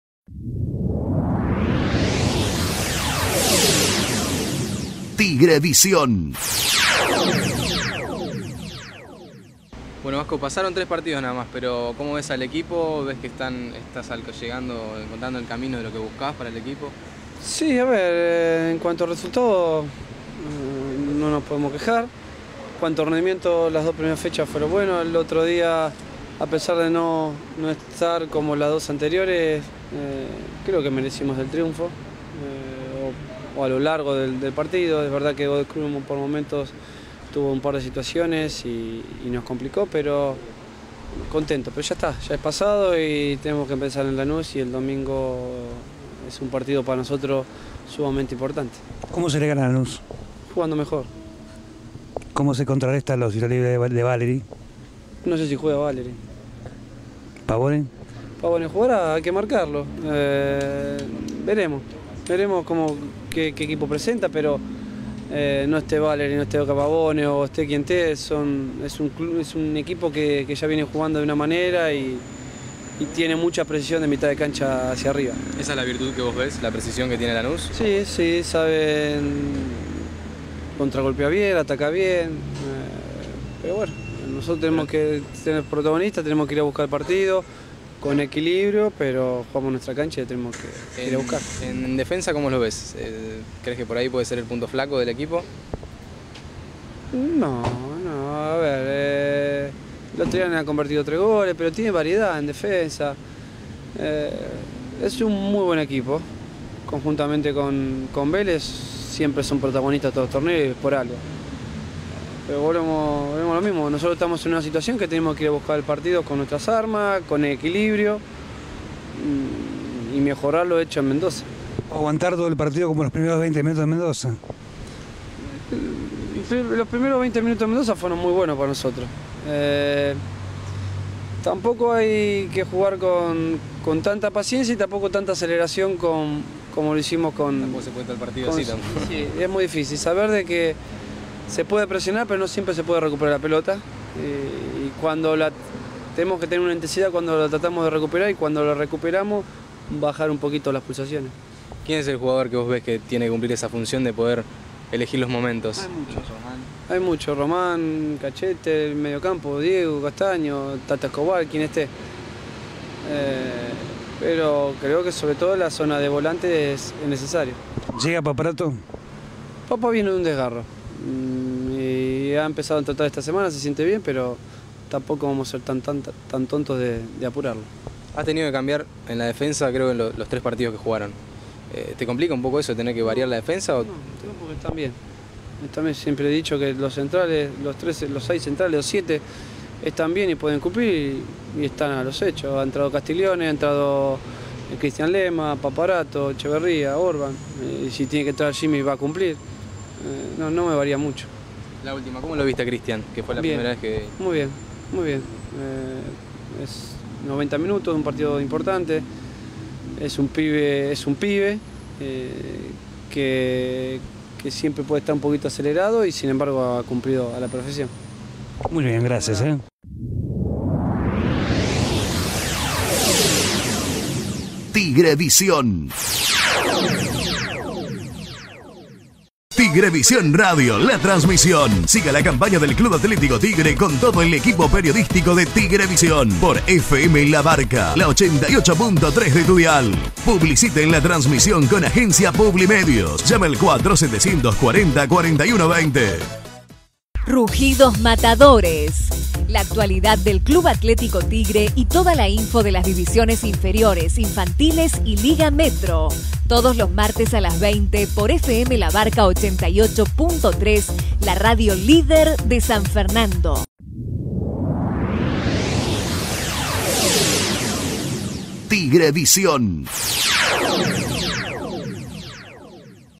TigreVisión TV: Nota a Rodolfo Arruabarrena
Mario Ocampo en la previa de Tigre - Lanús con el técnico del Matador: Rodolfo Arruabarrena